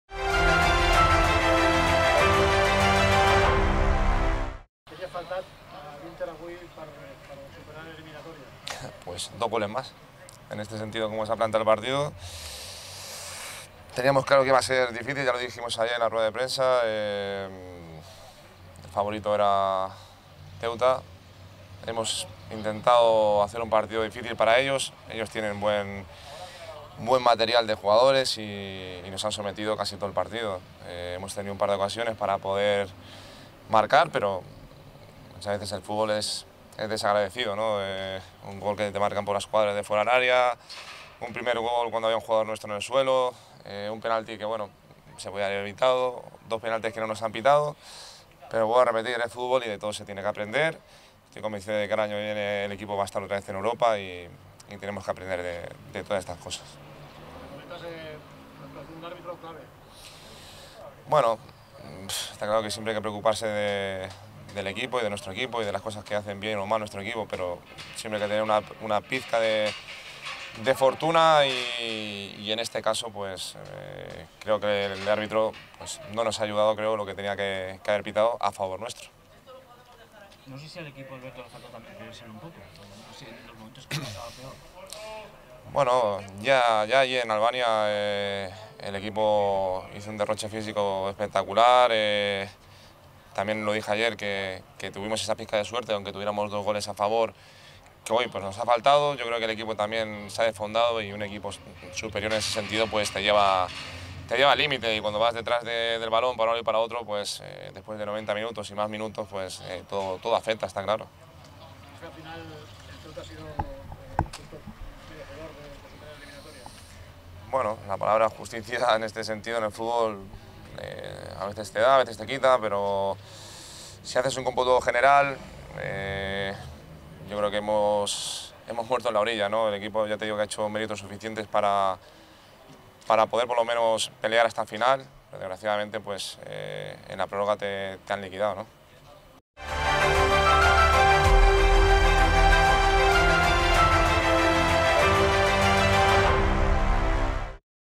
Alberto Lopo: "A vegades, el futbol és desagraït"
Roda de premsa d'Alberto Lopo després de la participació de l'Inter a la UEFA Conference League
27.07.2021